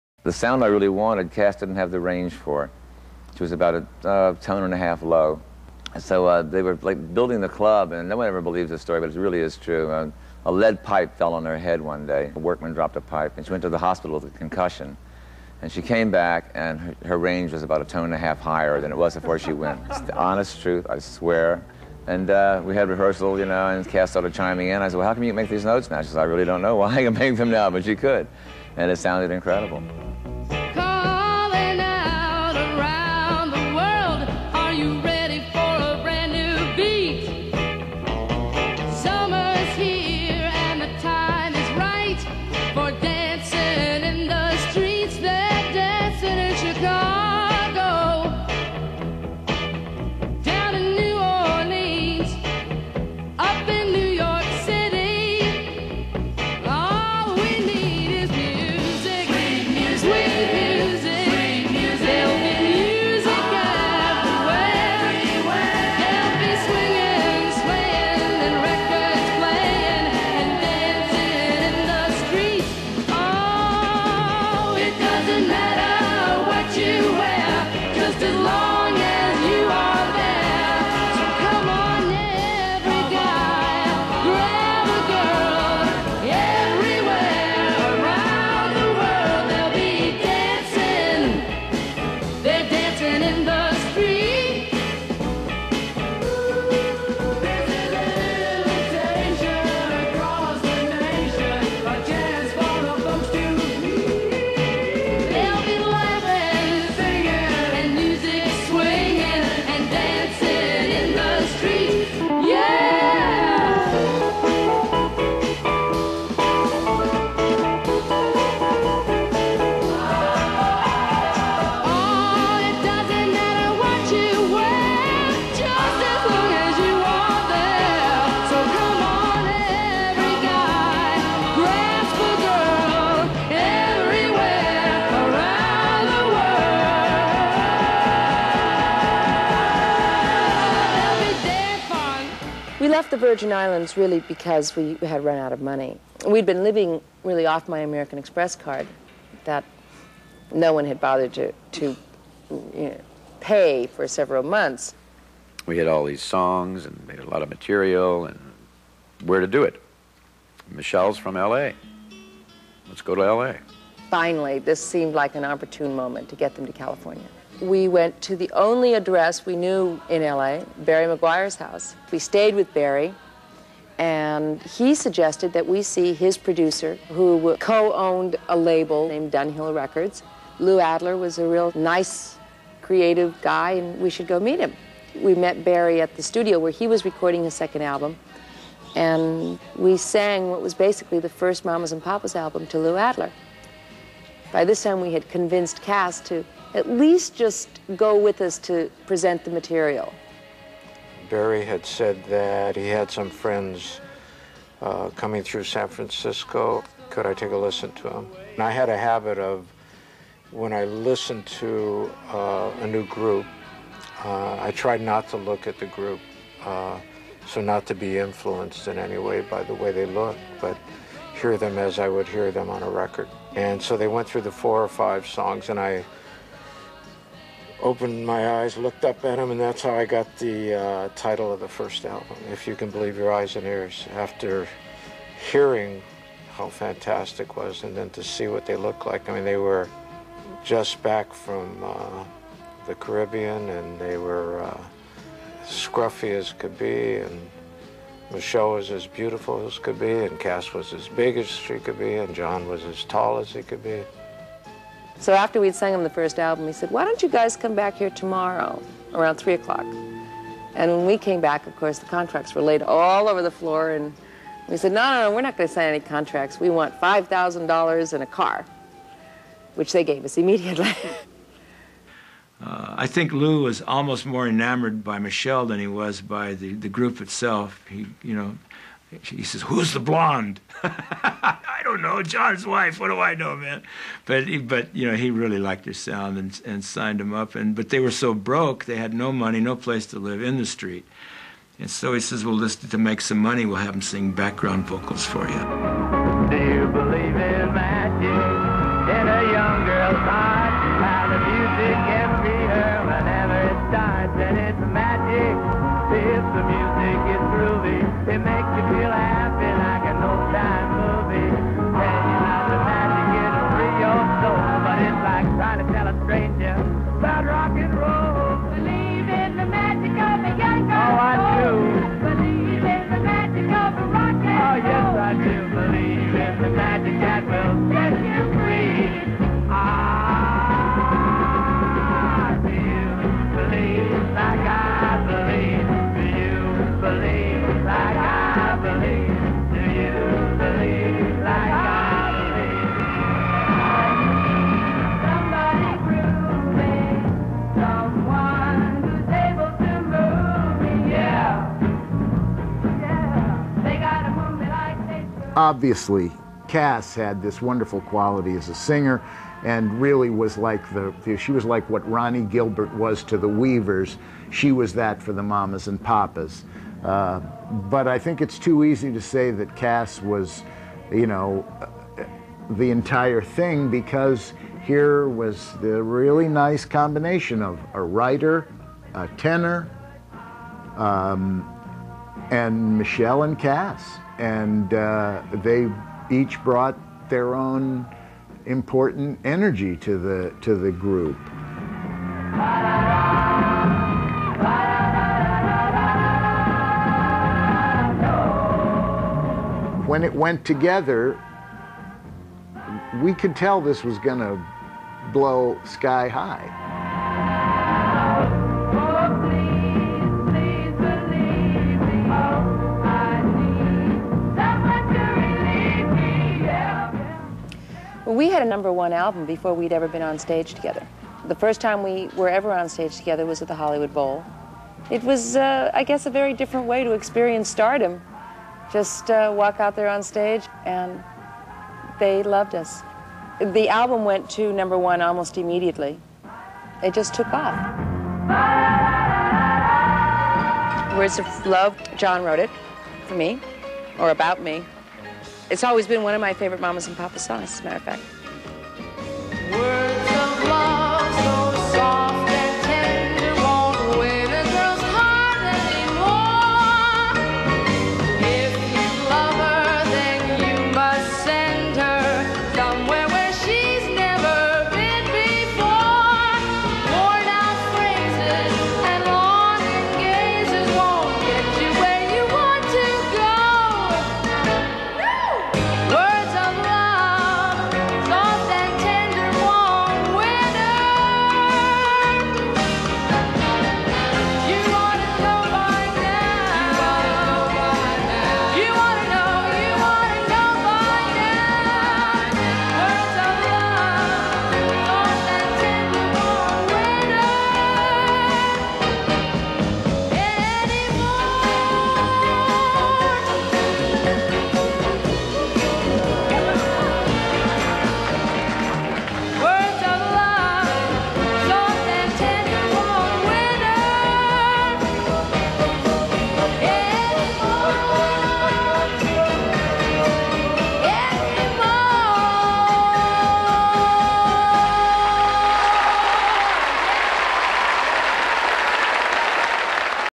MnP vid 2
The Mamas and the Papas. Documentary with music and photos.